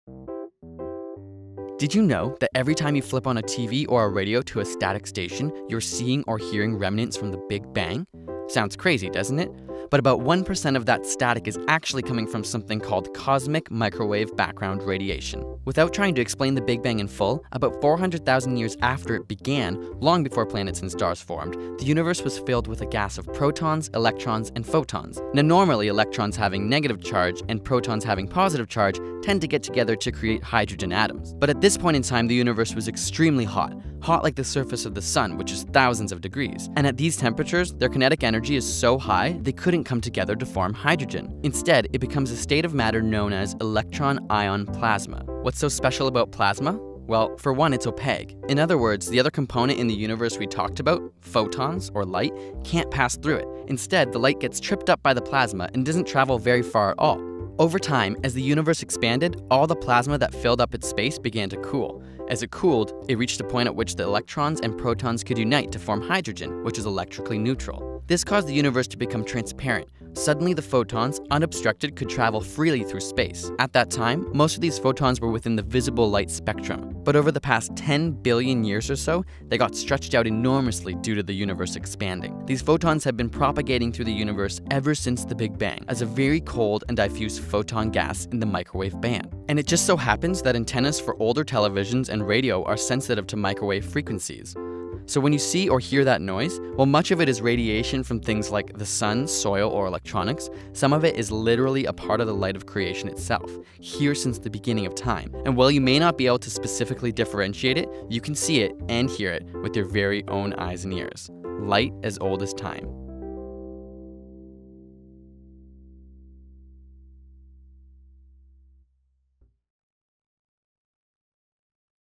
You Can See AND Hear The Big Bang
So you know a bit about the Big Bang - but did you know that you can physically see or hear it in your every day life? Find out how 'static' often contains remnants from this monumental event.

Written and created by Mitchell Moffit (@mitchellmoffit) and Gregory Brown (@whalewatchmeplz).